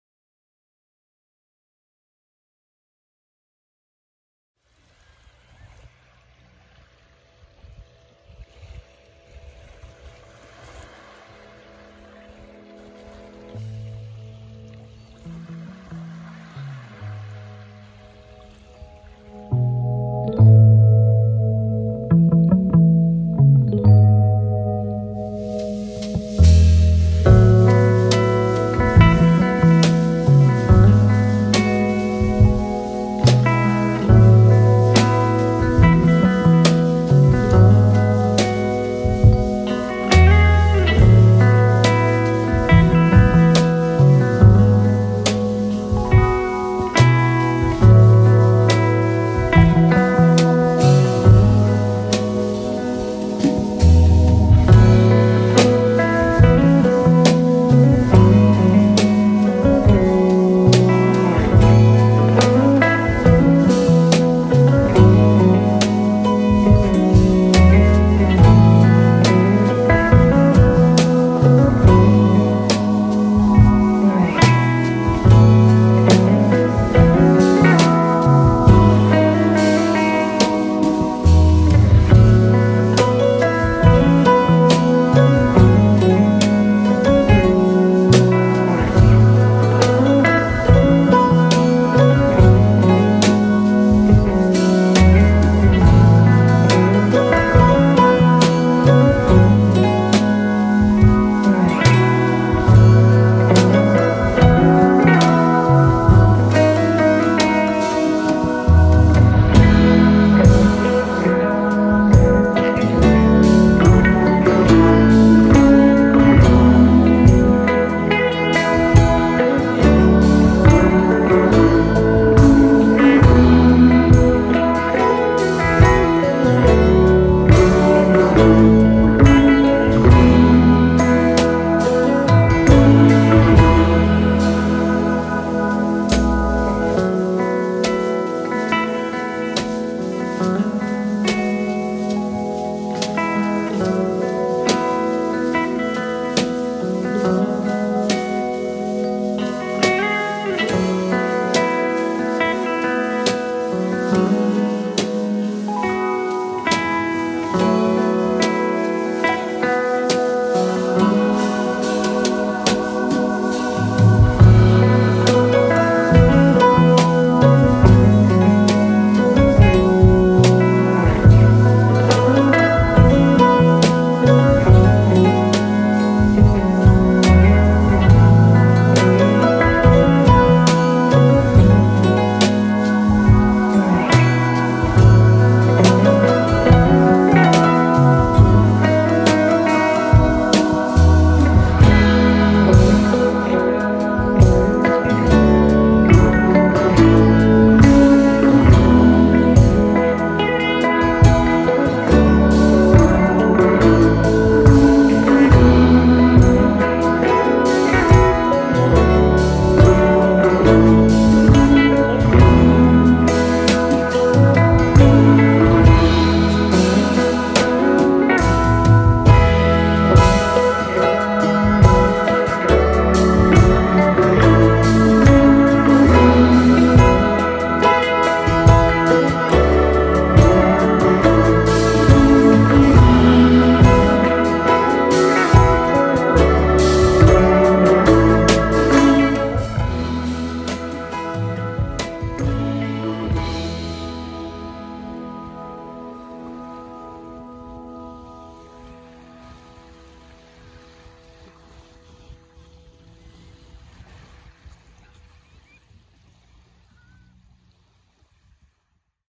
Beach Day! | 4k Cinematic | GoPro Hero 11 Mini
Took the quad and the trail truck to our favorite spot.